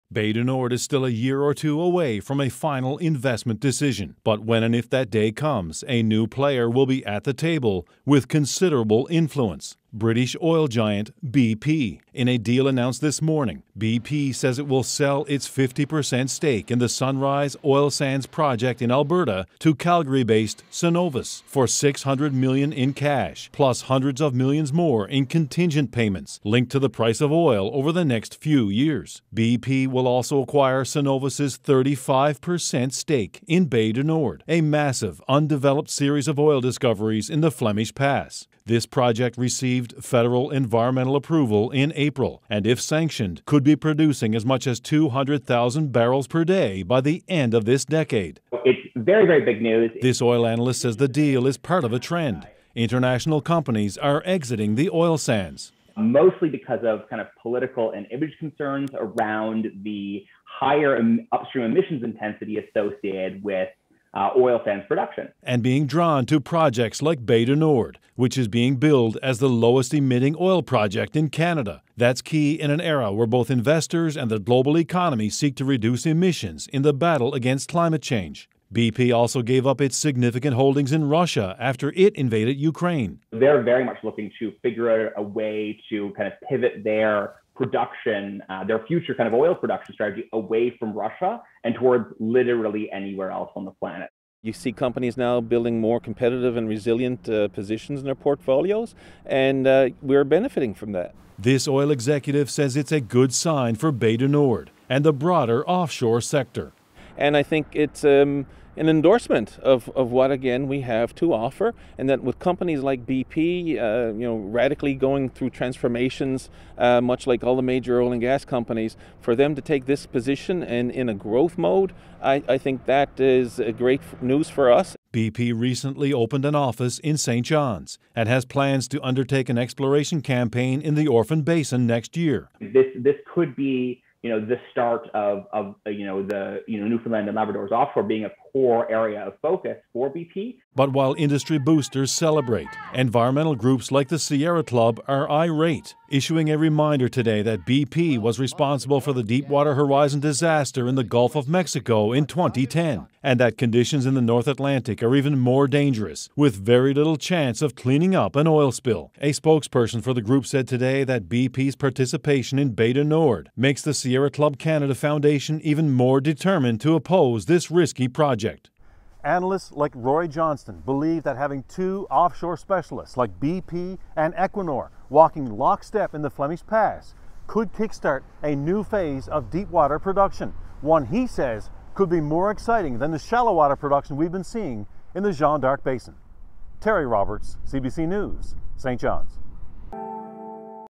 BP makes move into N.L. offshore with stake in Bay du Nord
Terry Roberts reports on a shakeup in the offshore oil industry, with BP picking up Calgary-based Cenovus's stake in the deepsea project.